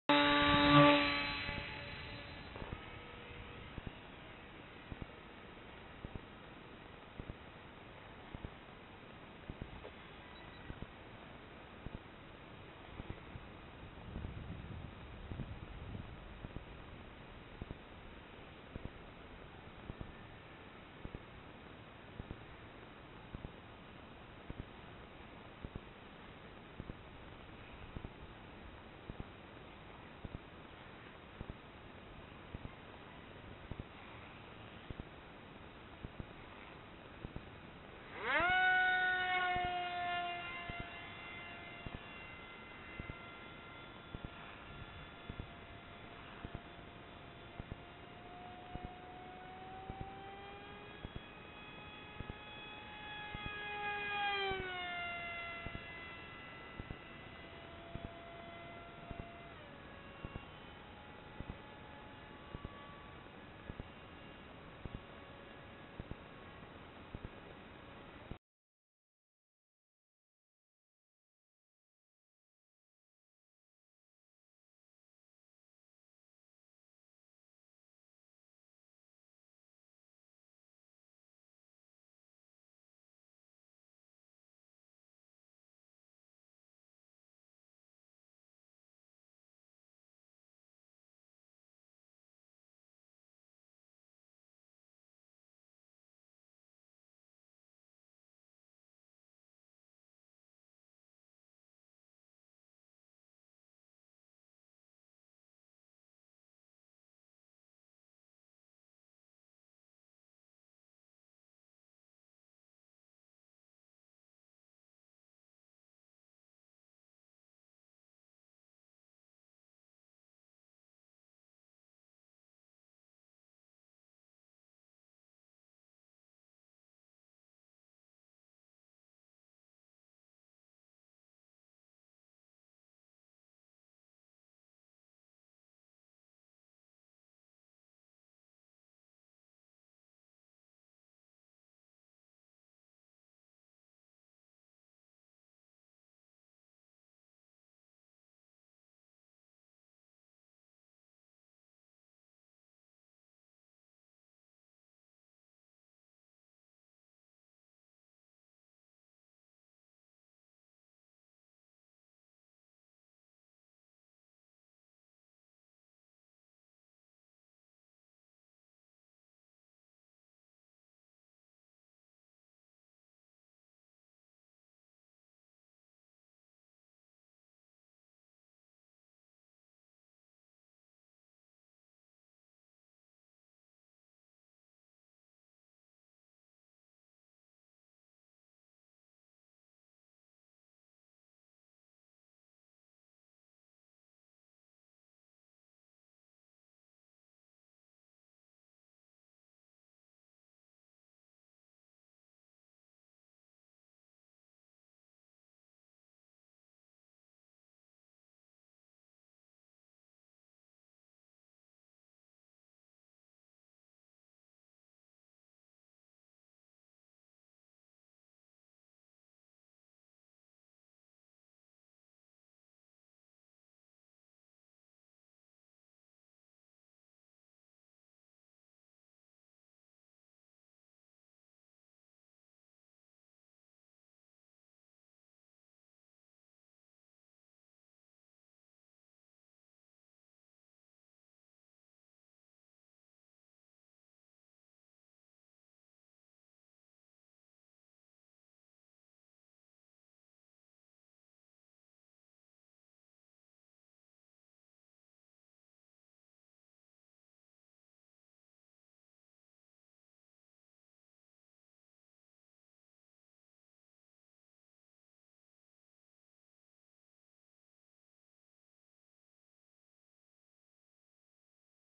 Hobby King Bixler 2 @ FD
Shot with HD Wing 720P

11.1v 2200 mah Battery, put straight into the nose, a little bit nose heavy.